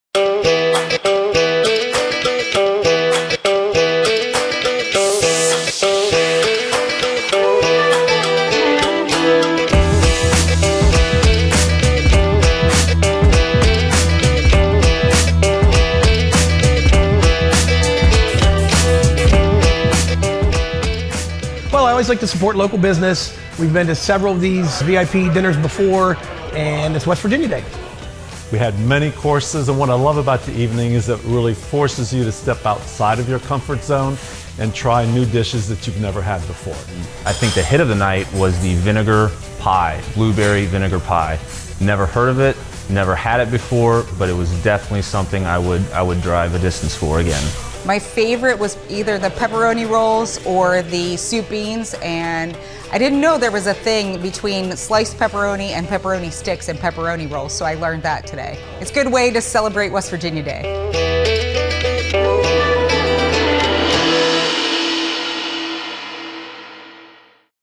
WV Whiskey Pairing Dinner | Vagabond Kitchen | June 20, 2022
We thoroughly enjoyed ourselves at our last Whiskey Pairing dinner held in honor of West Virginia Day, and we wanted to share some highlights from the night!

Thanks to everyone who came out and celebrated with us. We look forward to doing more of these in the future, and we hope you'ns invite your friends.

Menu:
► Still Hollow Corn Whiskey alongside traditional Coal Mine Pepperoni Roll
► Swilled Dog Straight Bourbon Whiskey and Soup Beans with chow chow and skillet cornbread
► Smooth Ambler Contradiction Bourbon with Hot Honey Fried Chicken garnished with fresh chives, with a side of coleslaw and pickled egg
► Isaiah Morgan Barrel Finished Rye with WV Beef Wellington made with biscuit dough and blistered green beans
► WV Fruit and Berry Bourbon with Vinegar Pie and blueberries